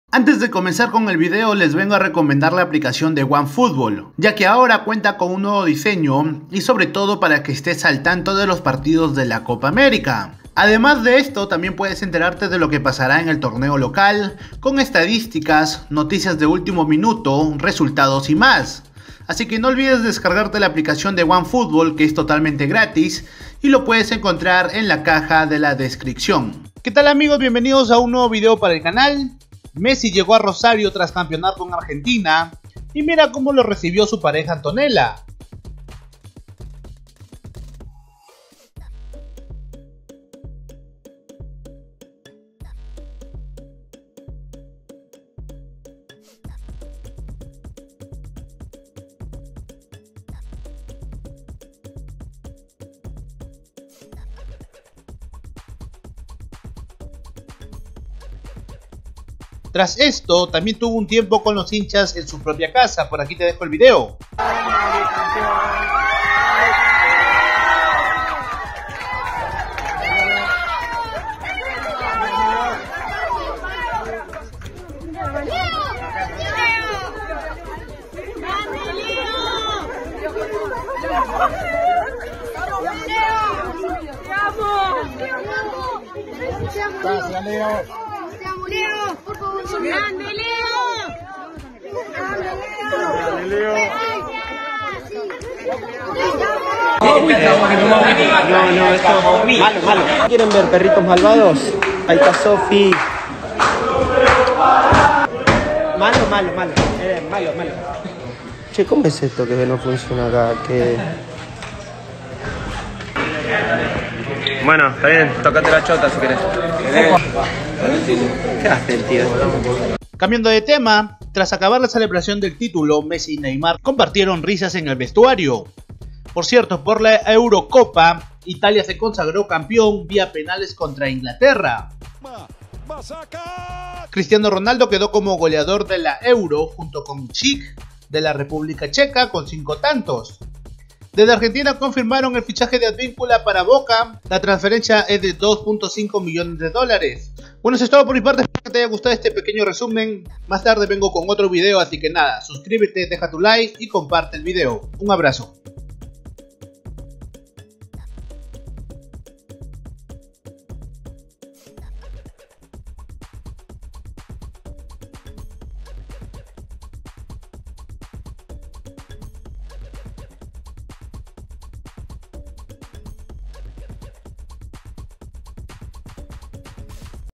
ITALIA es CAMPEÓN de EUROPA: VENCIÓ en PENALES a INGLATERRA | ADVINCULA ficha por BOCA JUNIORS y más
●En el video de hoy a conocer el penal fallado de Inglaterra y la obtención del titulo de Italia, lo que pasó con Messi tras campeonar con Argentina, el fichaje de Advincula a Boca y más!

●Apuesta en Meridianbet, utilizando mi codigo: 610714 en el siguiente link y obten S/. 40 para empezar a apostar
●Si quieres apoyar al canal: